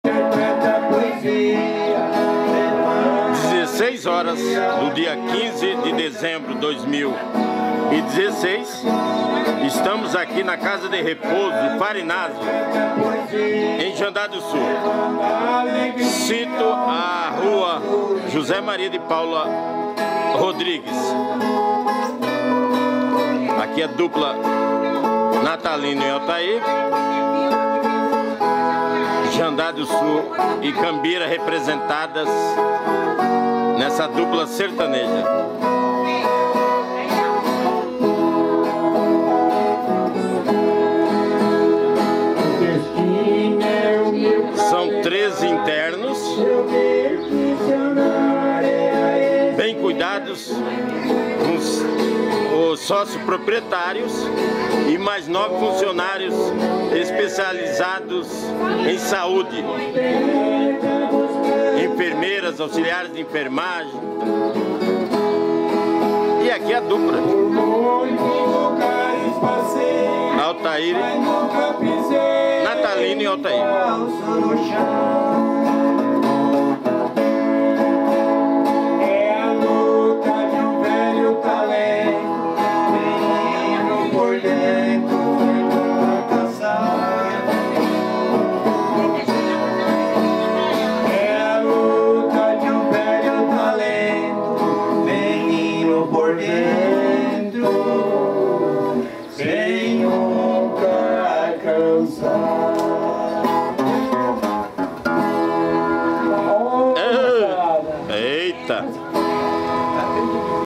00309 - Dupla Natalino e Altair na Casa de Repouso Farinazzo! p.1/2
Local: Rua José Maria de Paula Rodrigues, 303

Cidade: Jandaia do Sul - PR

Data: 15/12/2016.

A Dupla Natalino e Altair estiveram alegrando os internos (13), proprietários (02), funcionários (11), além dos visitantes nesta tarde de quinta-feira, a partir das 16 até as 17 horas.

Muitas músicas sertanejas, inclusive algumas compostas pela dupla.

Neste estabelecimentos o pessoal da 3ª Idade são bem tratados com pessoal especializados e treinados para as finalidades a que se dstinam!

E lá conhecemos o Senhor Dino. , italiano nato, que ao sairmos dizemos e fomos correspondidos:
 - Arreverdete...!

E ao que dissemos, agora, à distância:

"Felici Natali per tutti popolo del mondo!

Jandaia do Sul - PR, Dezembro de 2016.

a) Adiones Gomes da Silva (Ponga) - Pres. da SPJ e Delegado Cultural de Jandaia do Sul, via CONBLA/SP.